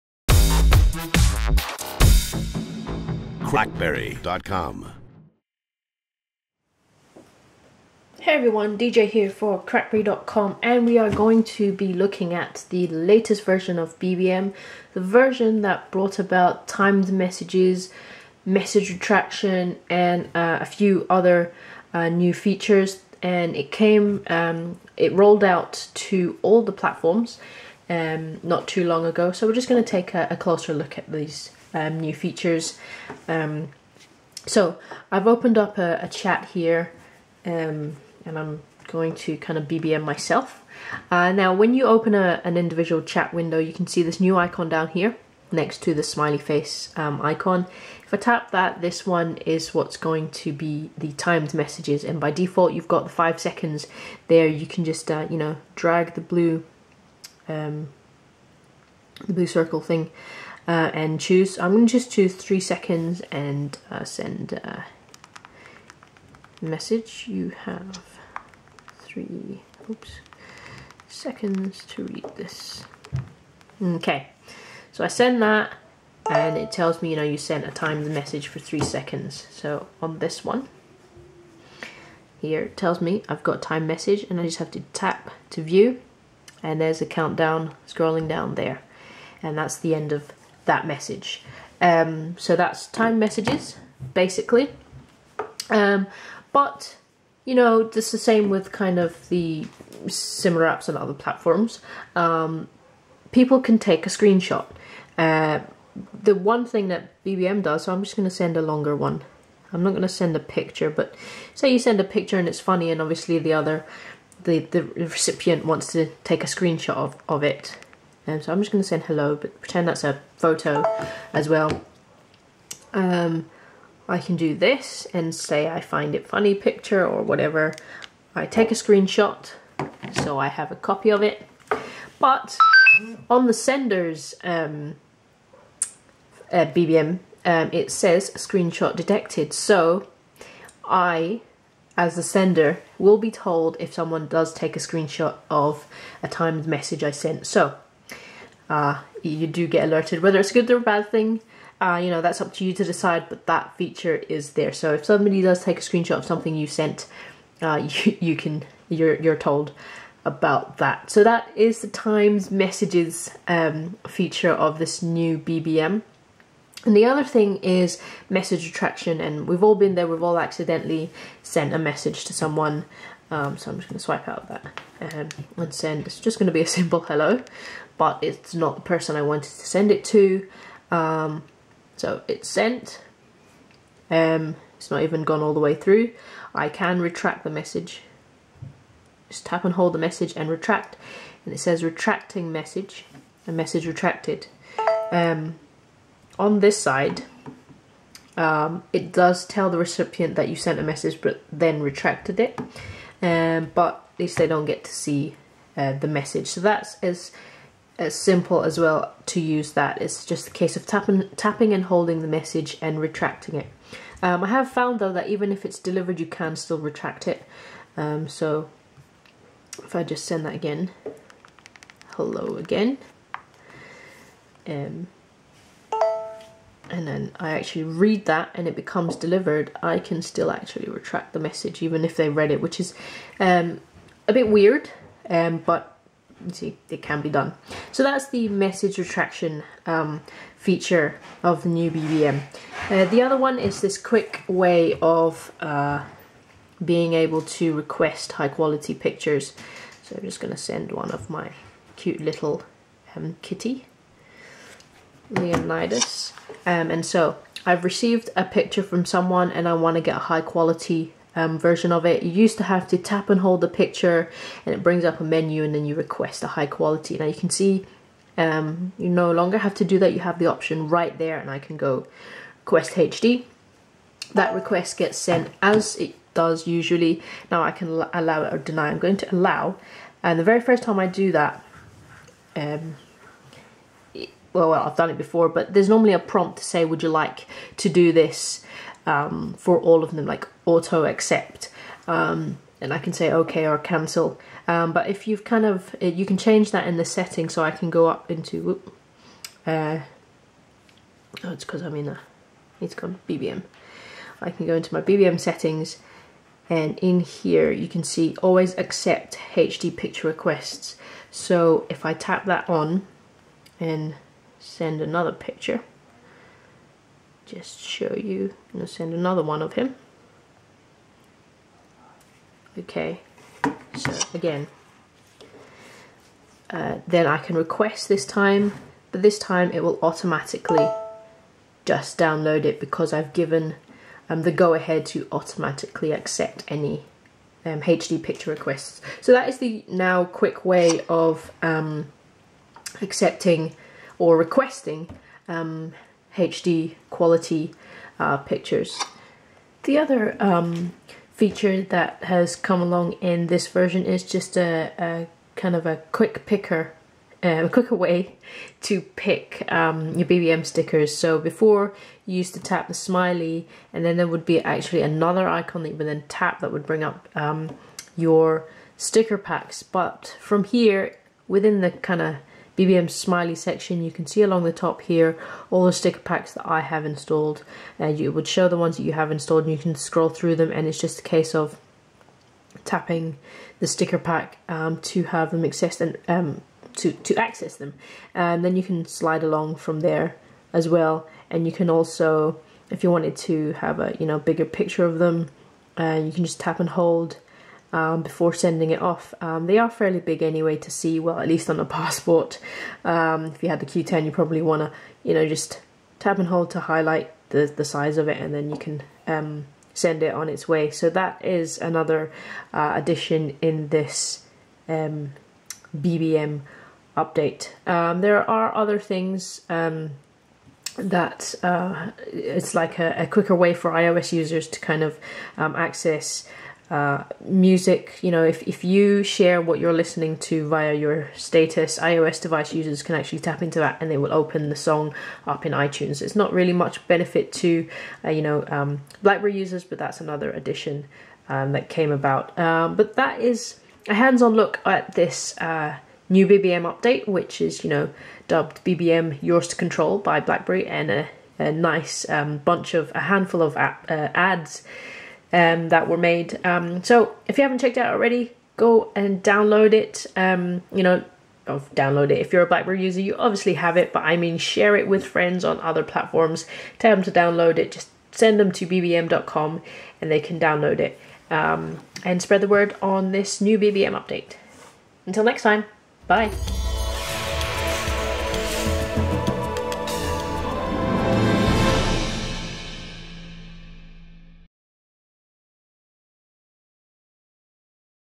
Hands-on with timed message and message retraction in BBM
BBM was recently updated to bring a handful of new features that includes timed messages, message retraction and more. We take a closer look at all the new features. Read more on CrackBerry.com.